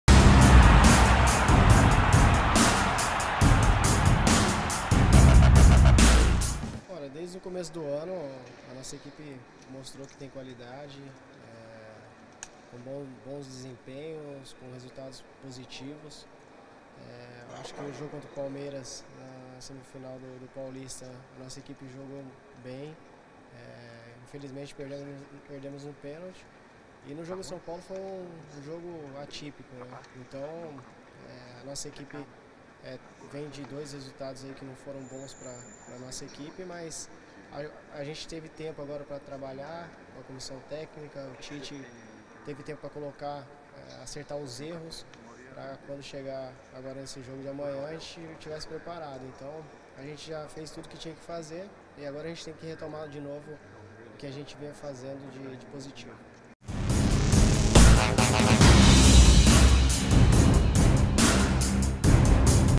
Jadson sobre o Corinthians: "Mostramos que temos qualidade"
O camisa 10 do Corinthians, Jadson, falou sobre o confronto de amanhã contra o Guaraní.